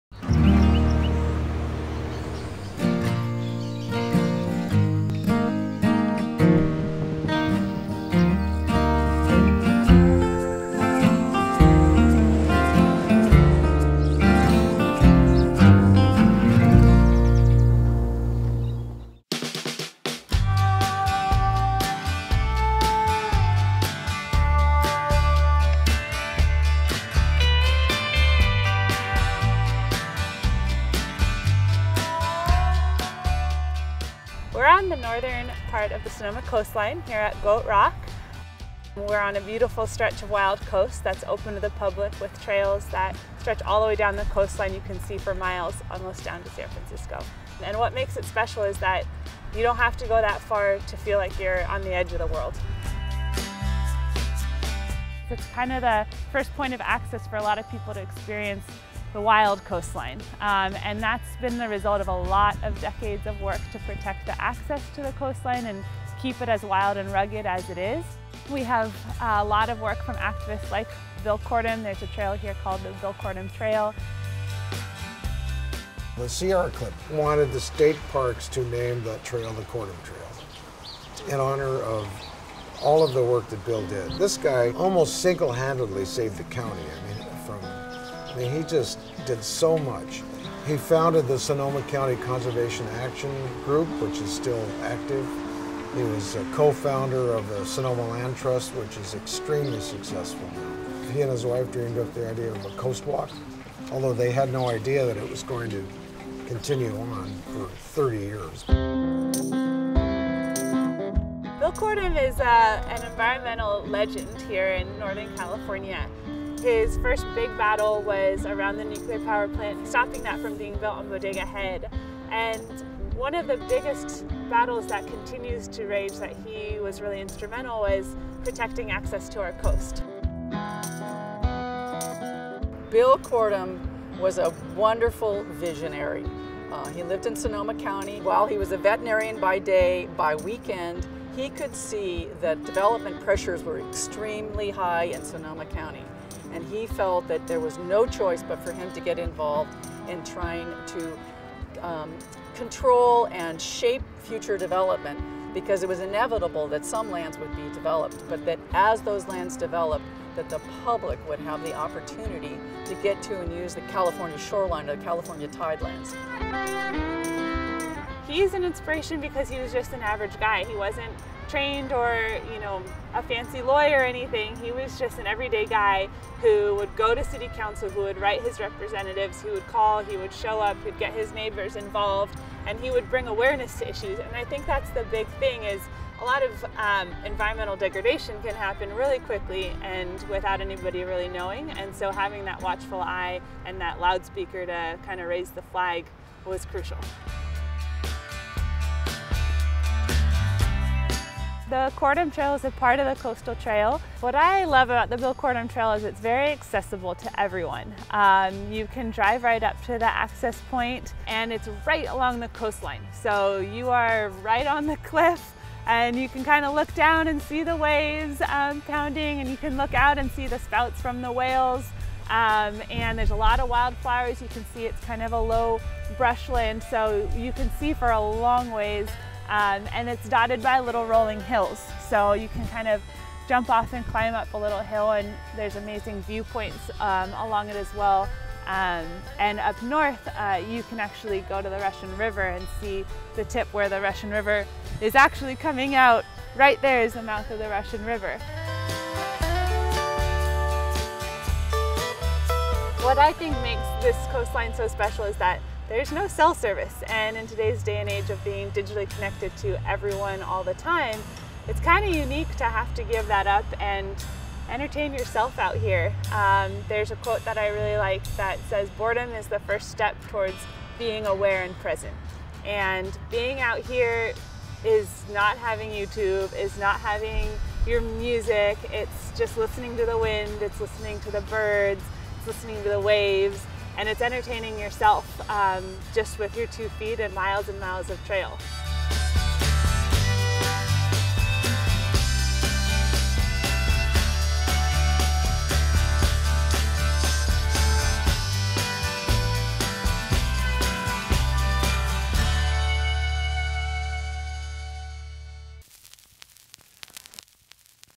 California Coastal Trail - Kortum Trail, Sonoma County
California Coastal Trail video by Rigler Creative, highlighting the Bill Kortum Trail in Sonoma County.  Produced with a Whale Tail grant from the Coastal Commission to the International Documentary Association.